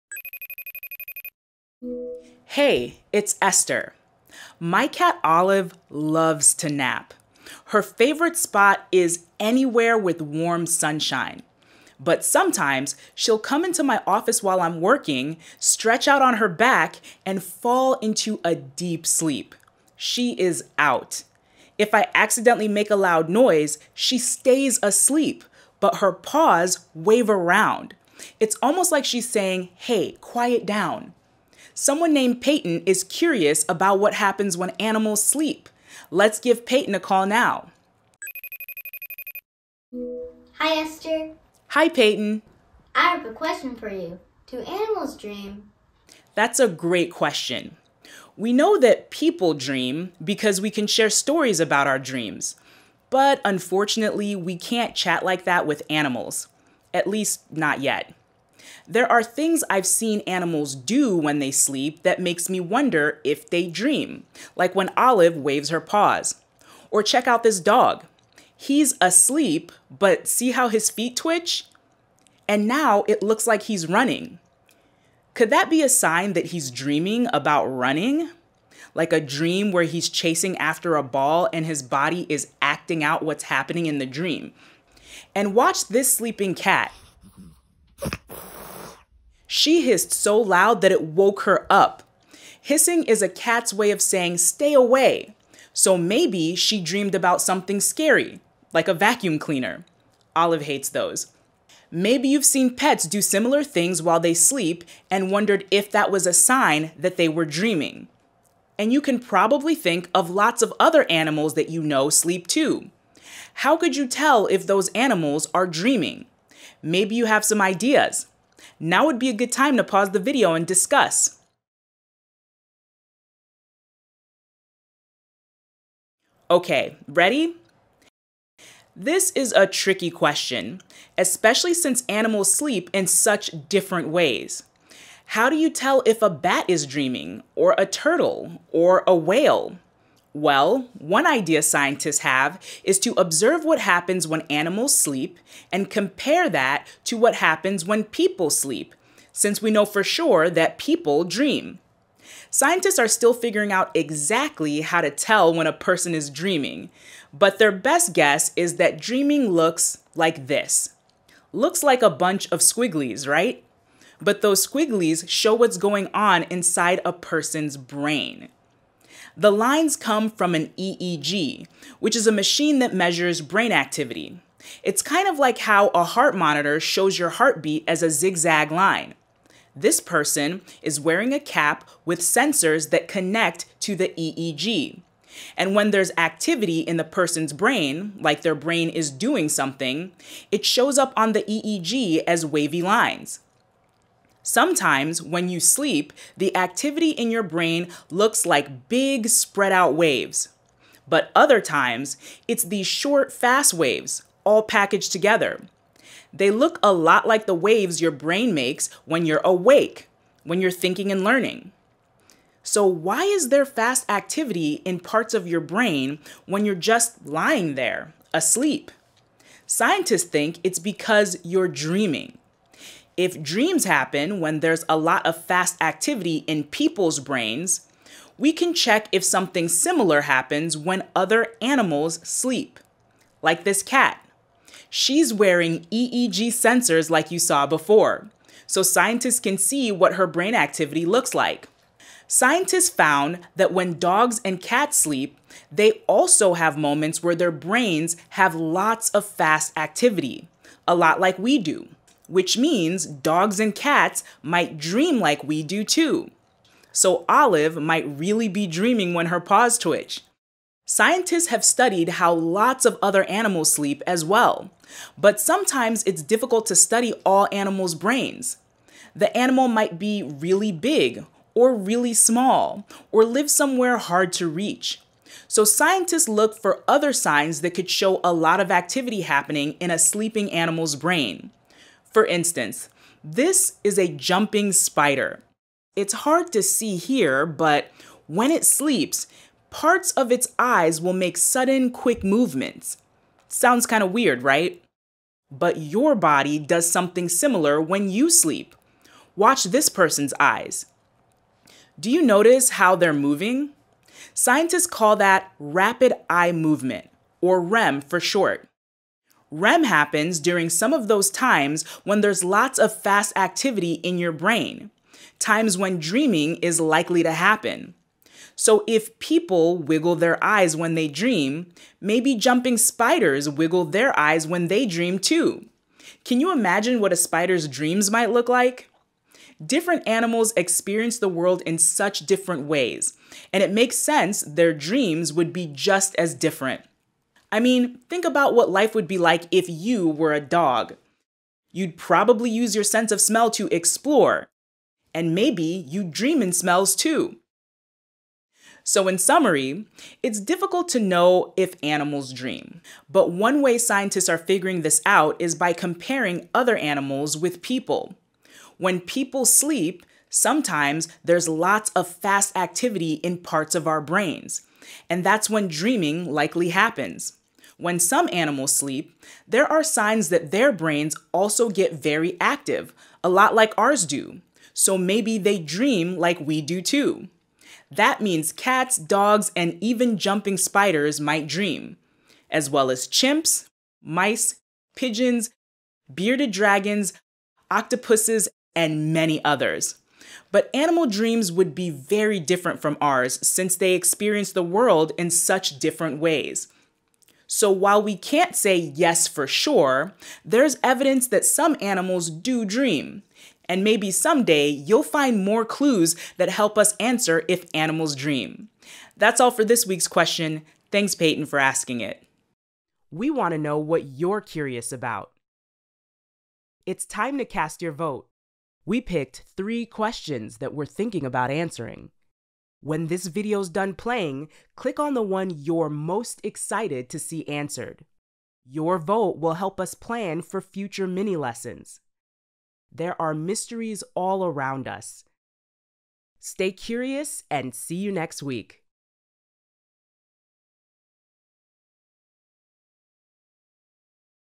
Do animals dream?
Are your kids wondering, “Do animals dream?” This question came from Peyton, a student from the United States.